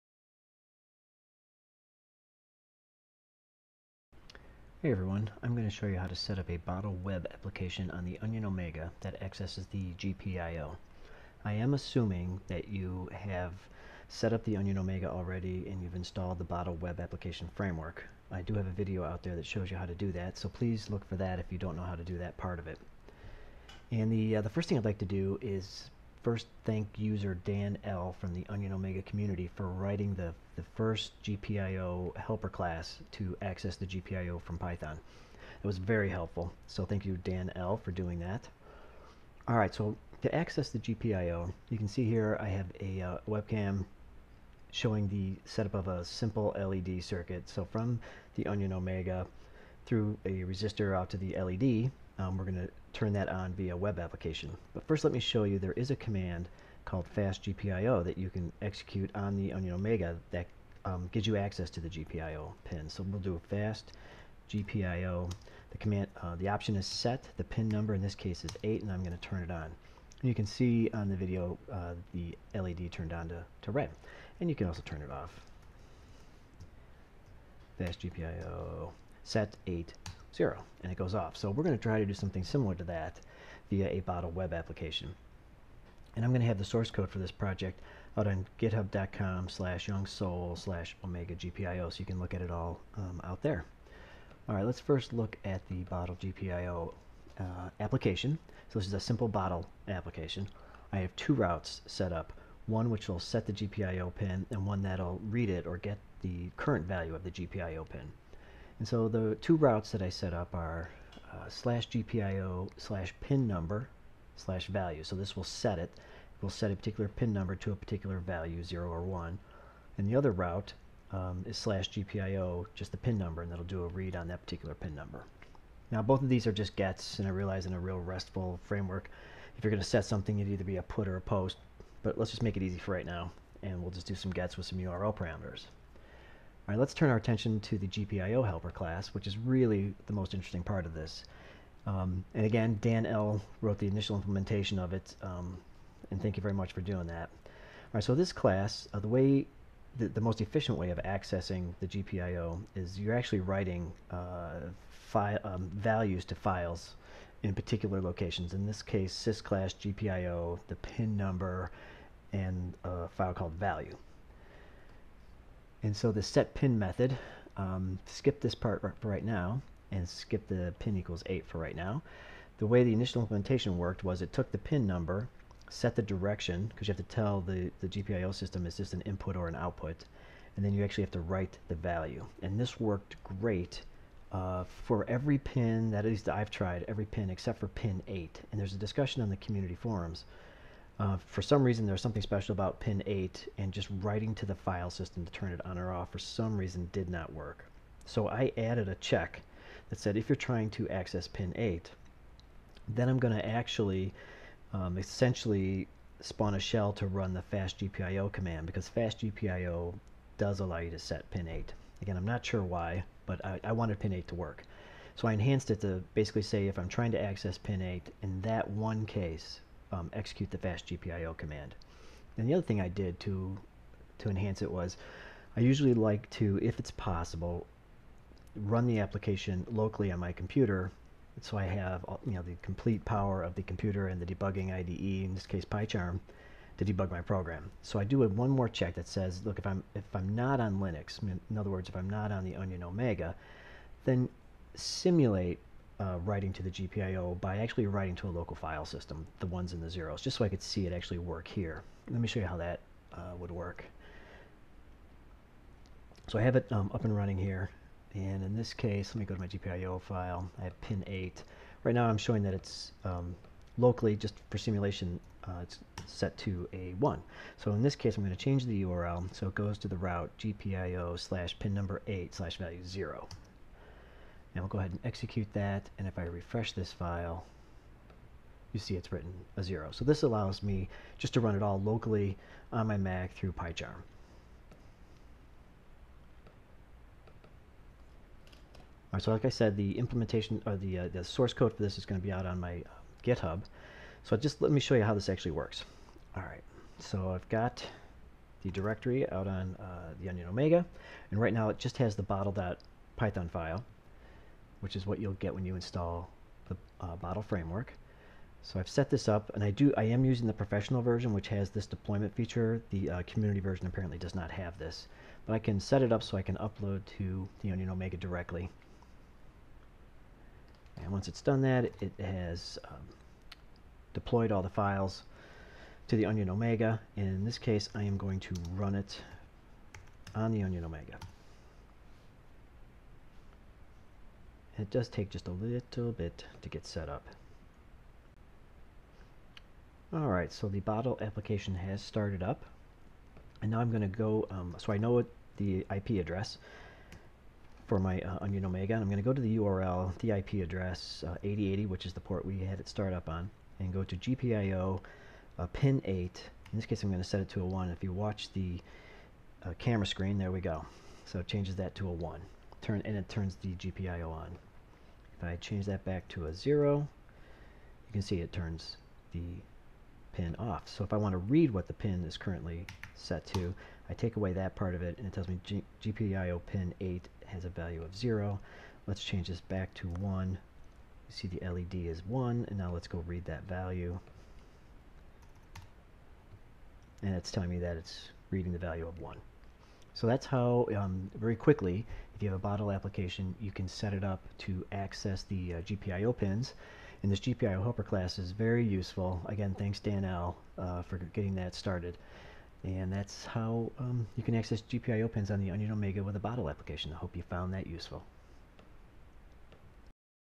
Access GPIO on Onion Omega with Bottle Web App Framework
How to access the GPIO pins of the Onion Omega using the Bottle web application framework.

Credits:
Onion Omega User Dan L wrote the first GPIOHelper.py implementation.